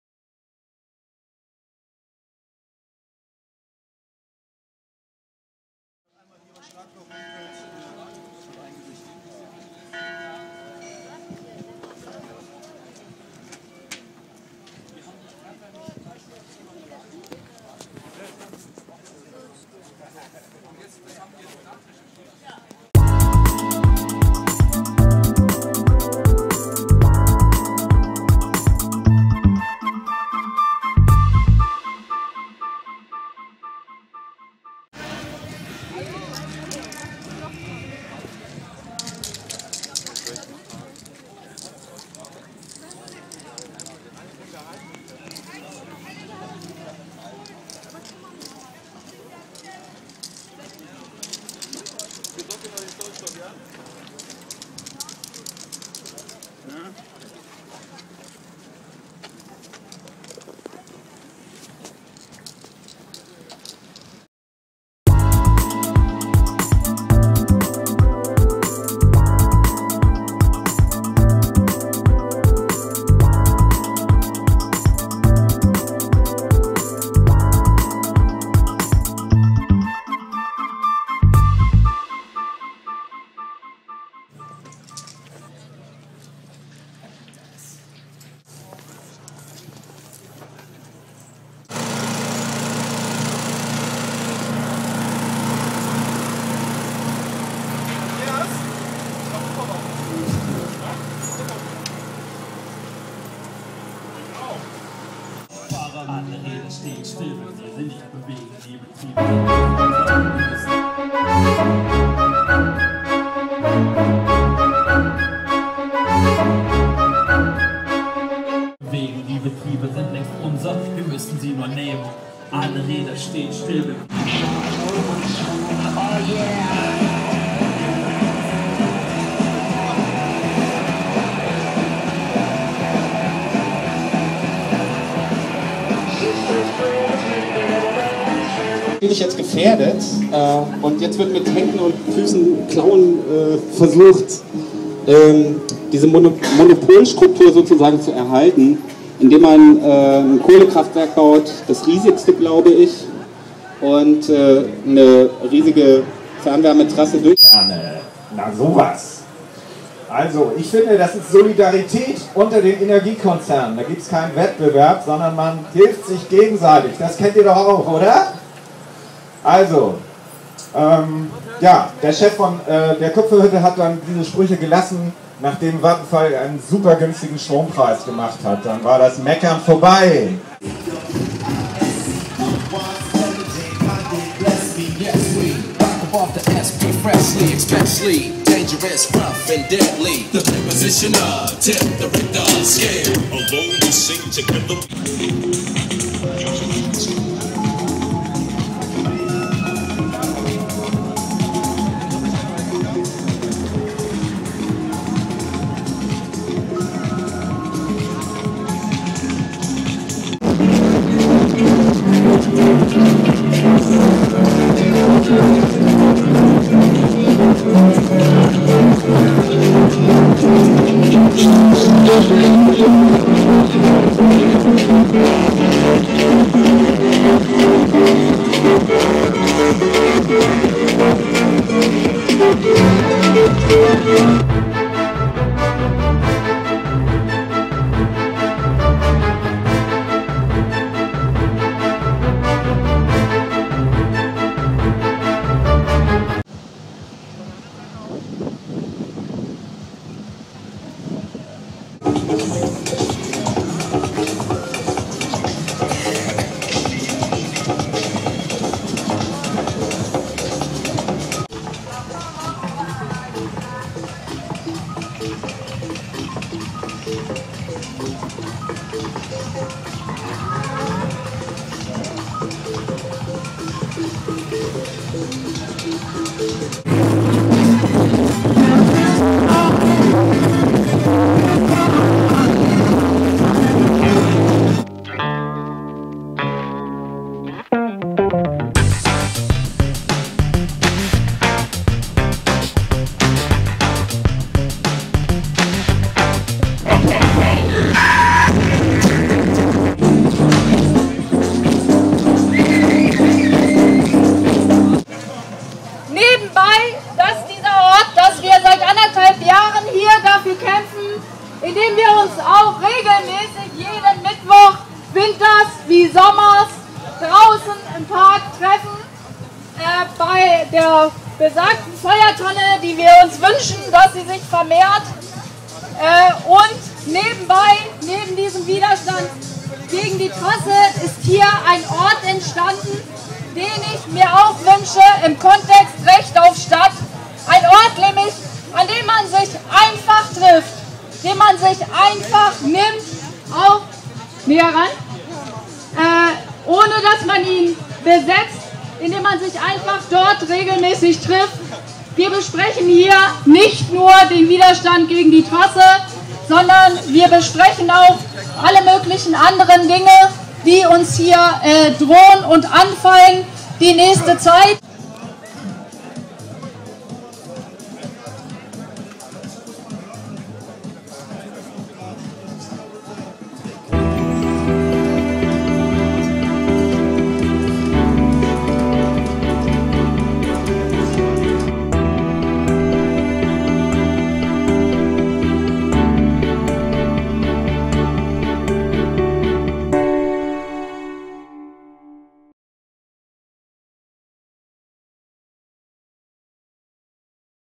Vattenfall wegbassen
Die Hamburger Sektion der Hedonistischen Internationale übermittelte uns folgenden Aufruf:

Parade Vattenfall WEGBASSEN!

Treffpunkt: 05. Juni 2011, 13:00 Uhr, Park Fiction
Auftaktkundgebung: 13:30 Uhr
Abfahrt: 14.00 Uhr

Wir bassen da, wo Vattenfall baggern will -- Route:
Bernard-Nocht-Str. -- Davidstr. -- Reeperbahn -- Holstenstraße -- Suttnerpark -- Lunapark.

Wagen 1 -- Moorburgtrassen Initiative (Rednerwagen)

Wagen des Bündnisses der Hedonistischen Internationalen
Wagen 2 -- elbrausch/Rotzige Beatz/Like Birdz/ILL
Wagen 3 -- Bananenrepublik/Grasgeflüster